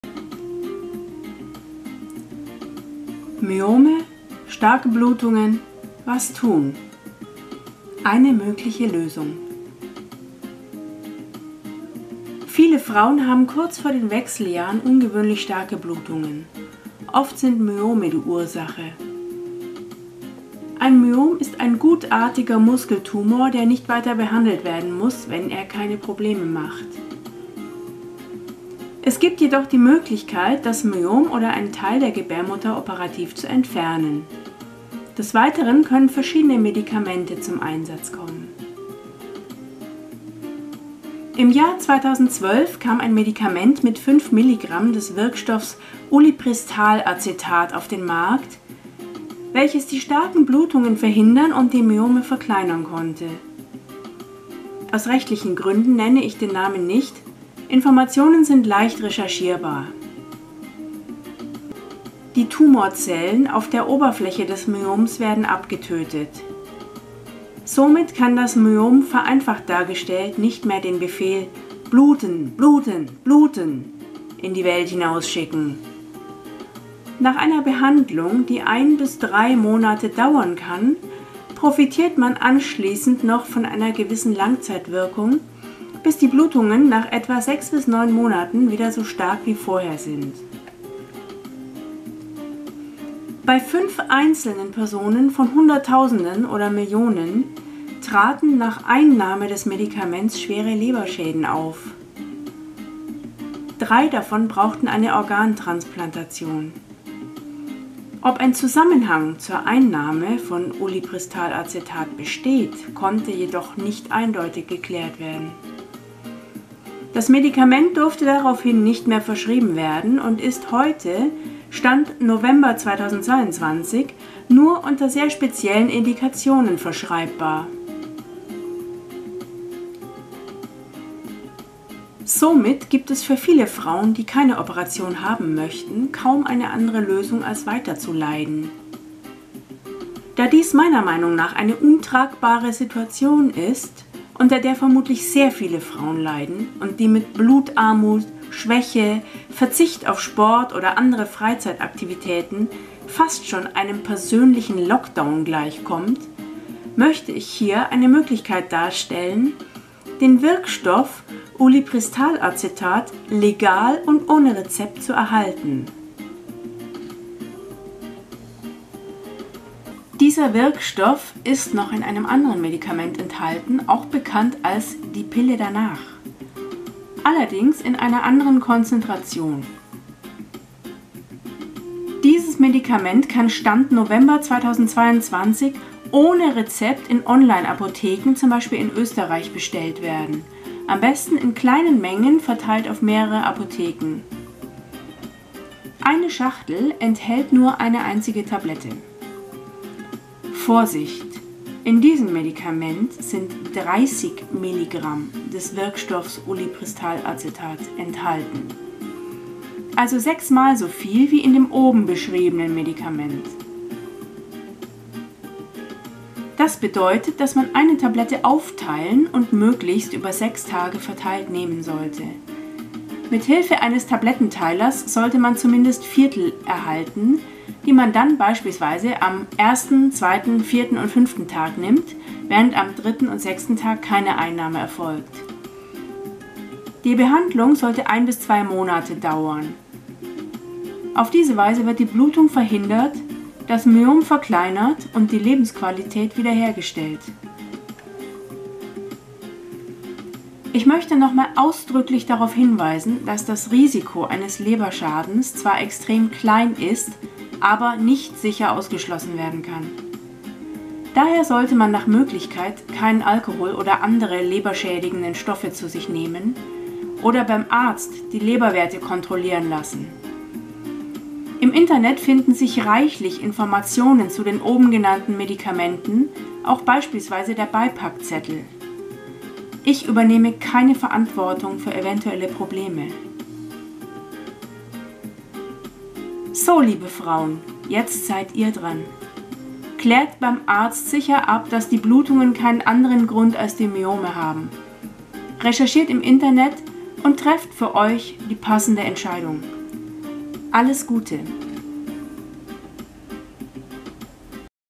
Myome, starke Blutungen, was tun? Eine mögliche Lösung
Viele Frauen leiden kurz vor den Wechseljahren an Myomen und starken Blutungen, wodurch die Lebensqualität beeinträchtigt wird. Eine mögliche Lösung wird hier gezeigt.